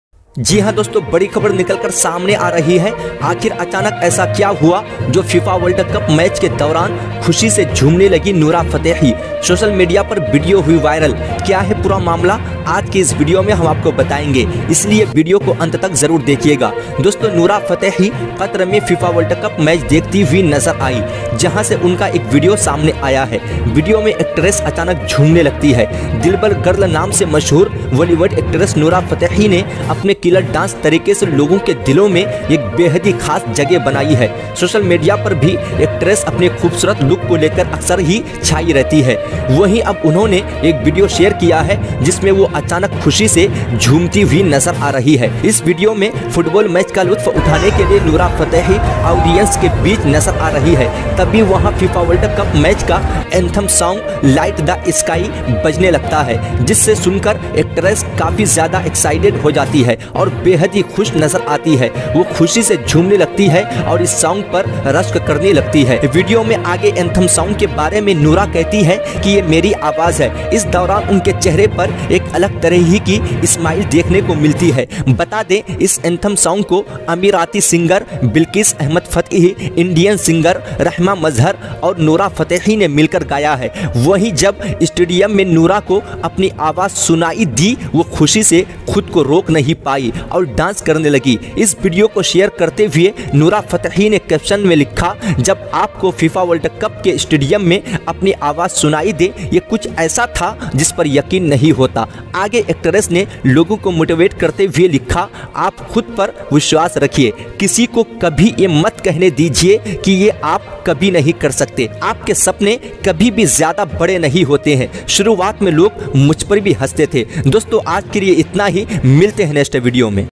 फीफा विश्वकप में नवरा फतेह- विश्व कप में डांस करने लगी नोरा फतेही _Fifa world cup.Nora Fatehi
फीफा विश्वकप में नवरा फतेह- विश्व कप में डांस करने लगी नोरा फतेही वीडियो हुई वायरल

video highlights

नवरा फतेह ने किया डांस
फीफा विश्वकप में नवरा फतेह
Nora fatahe ka dance
bollywood star nora fathe